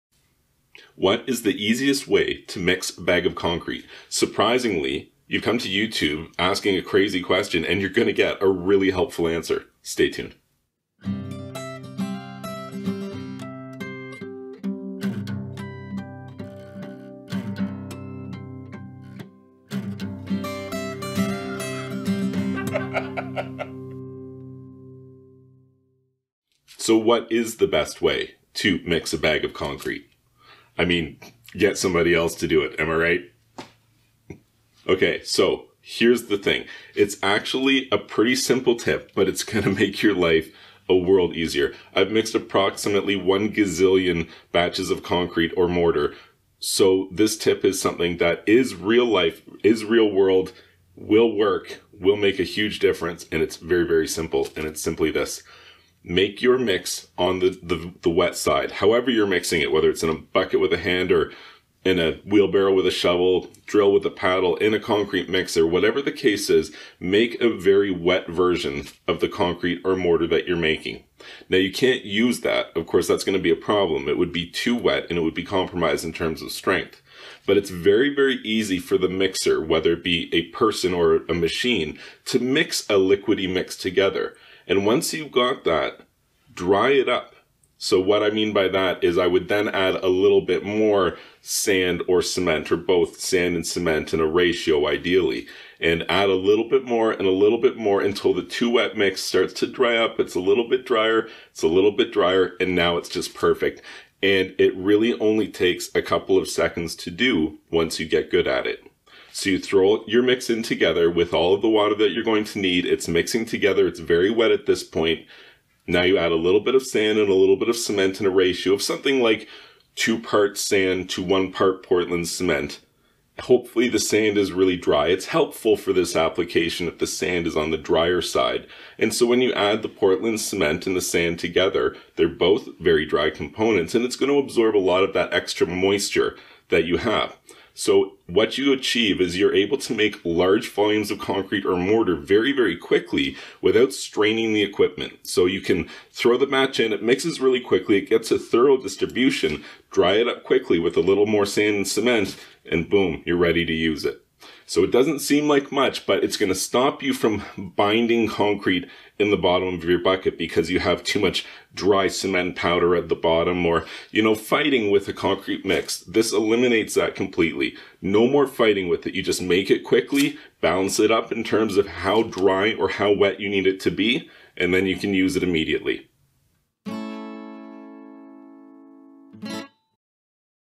What Is The Easiest Way To Mix Concrete?
A great tip for the easiest way to mix concrete is to simply start by making the mix wet.  Add more water and mix together well to make sure that the dry ingredients are thoroughly combined, whether it is a bag of premixed concrete or your own concrete mortar recipe of sand and cement, and then add more of the dry components to use up that extra water.  Mixing by hand or with equipment like a drill and paddle or concrete mixer, it much easier to mix the batch of concrete or mortar when it is runny or watery.  Slowly add more of the dry ingredients to the mixture in the original recipe ratio (for example 2 parts sand to 1 part portland cement) to dry up the mix to the desired consistency while still keeping it much easier to get an even distribution of those ingredients throughout and not be left with dry cement or sand at the bottom.

It may seem like a really simple tip but it really does help avoid any binding of the concrete ingredients or having a bunch of dry stuff left in the bottom of your bucket or mixer.  It also allows you to make large volumes of concrete or mortar very quickly and mixed thoroughly without straining the equipment or yourself.  So starting with a wetter concrete or mortar mix and then adding more dry ingredients makes it much easier on you and the equipment used and avoids some of those common concrete mixing problems.